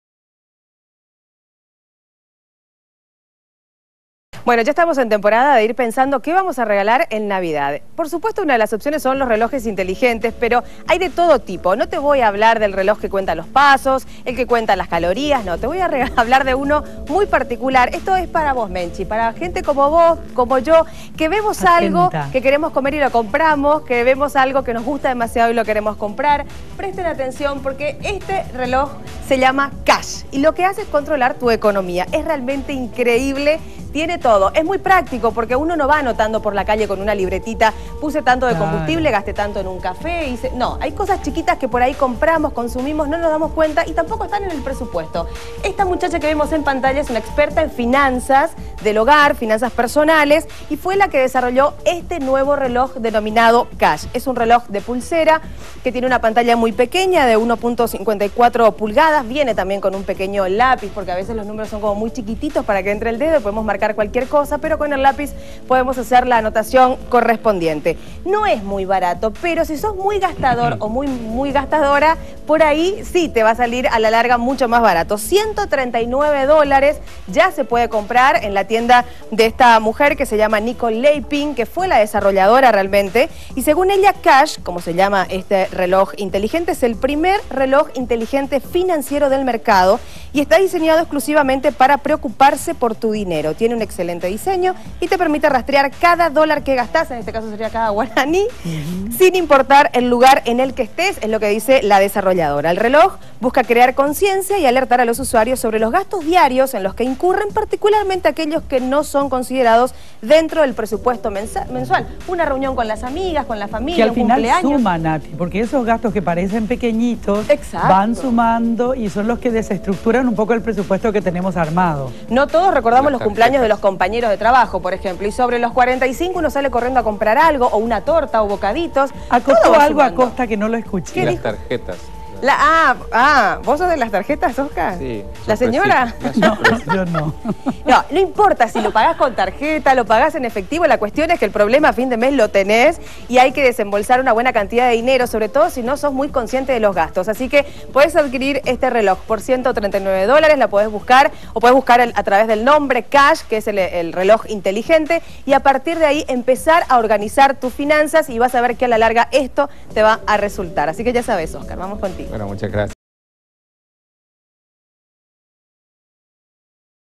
"Cash" es el reloj inteligente que te dice cuánto gastas - 12/12/2014
En este segmento de tecnología te treamos a "chash", el reloj inteligente que te dice cuánto gastas.El reloj busca crear conciencia y alertar a los usuarios sobre los gastos diarios en los que incurren.
Telediario